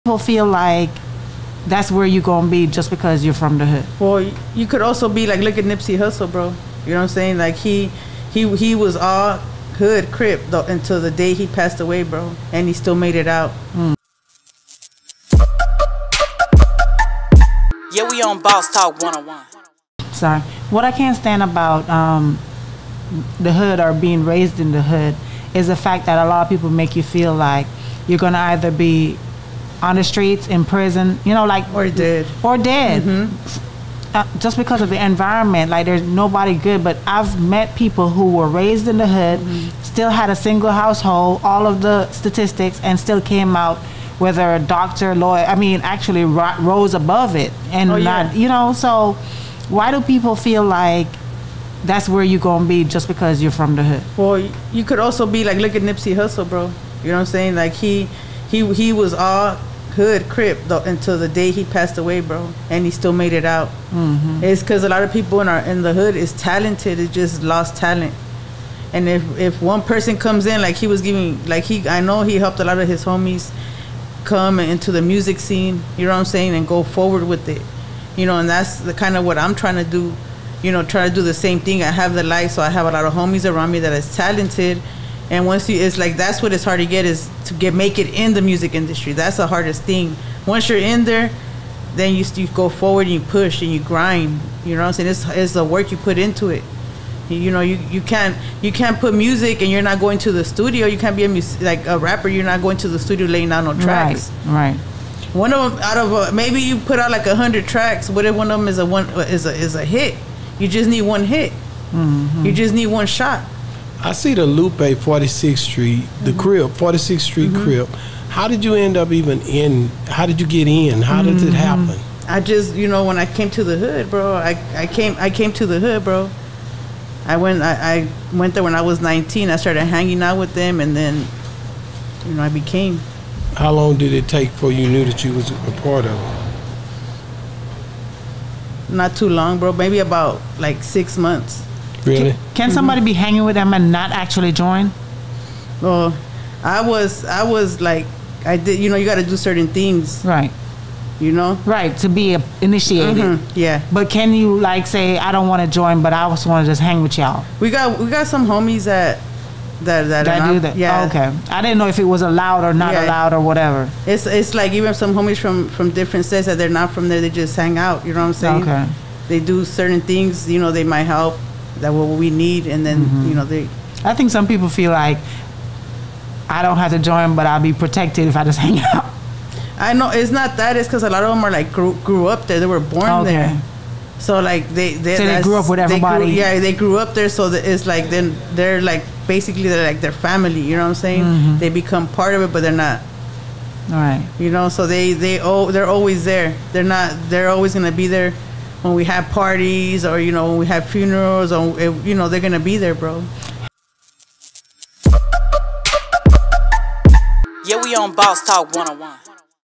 Lupe/ Crip Mac on Nipsey Hussle Crip Until He Died & How She Became 46 Crip | Free Crip Mac (Part 5)
Lupe/ Crip Mac on Nipsey Hussle Crip Until He Died & How She Became 46 Crip | Free Crip Mac